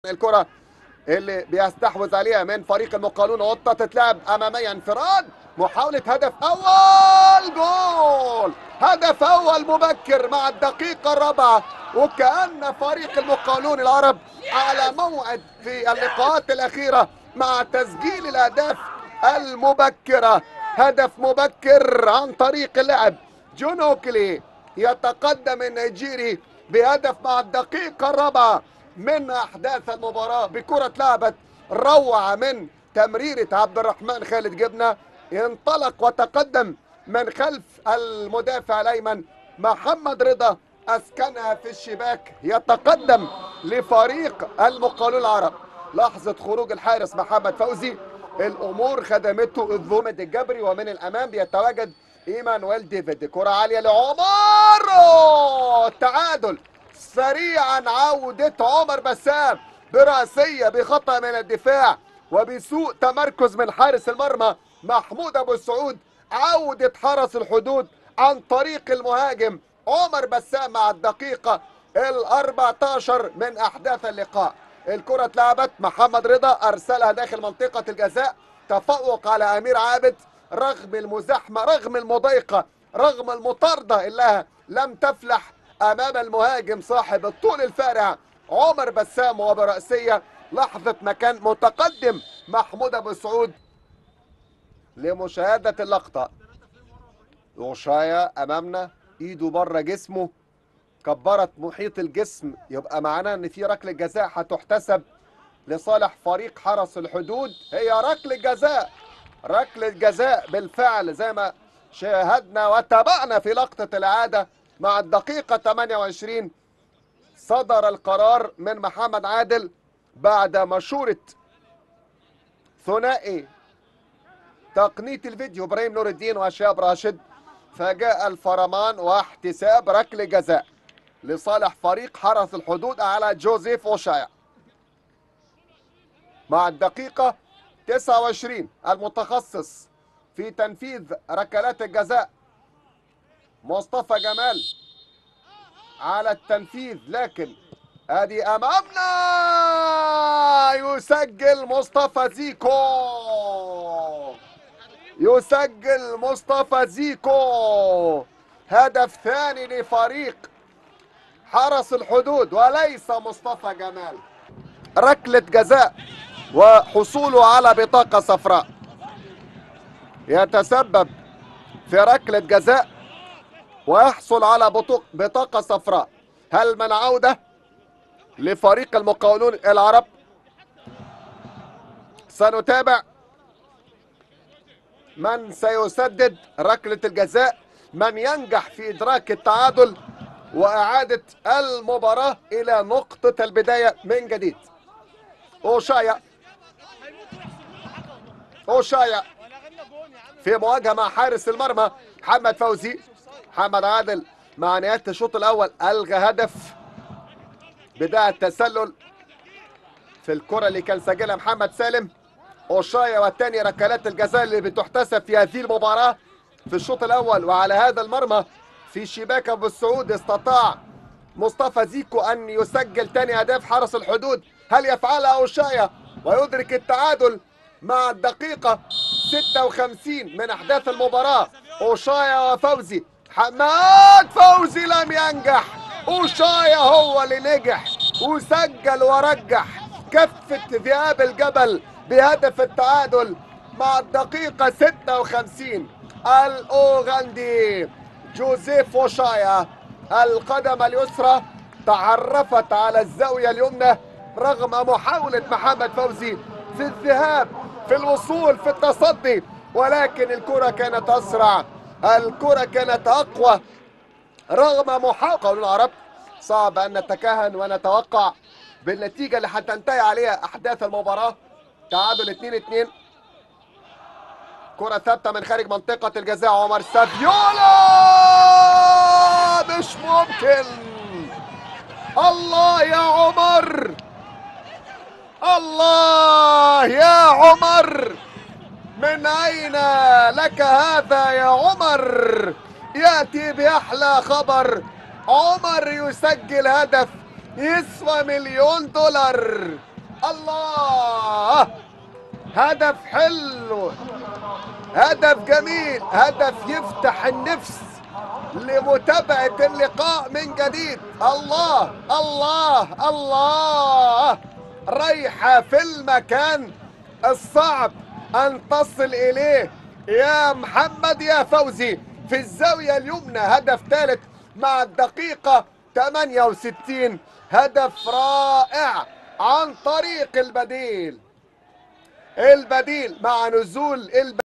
الأهداف الكاملة لمباراة حرس الحدود والمقاولون العرب (2-3) في الدوري العام
هتقدر تشوف كل وأهم لقطات الدوري المصري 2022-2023